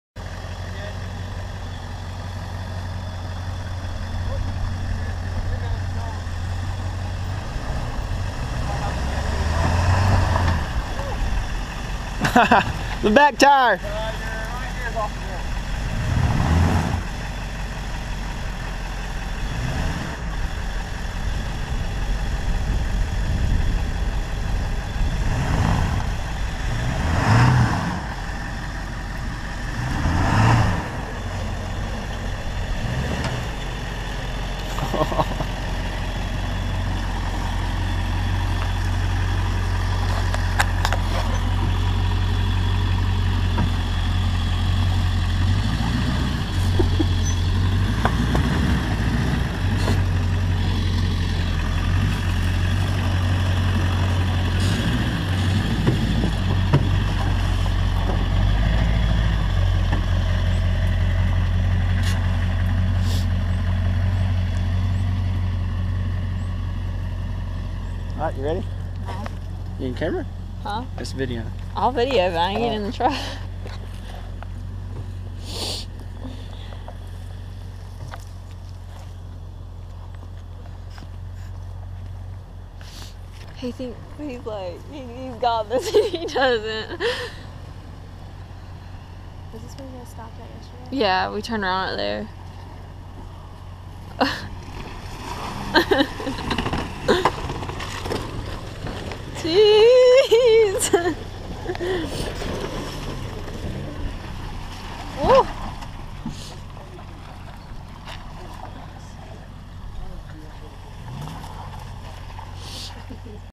Trail Riding N GA Hiawassee area
F-250 and Tacoma out trail riding . No lockers.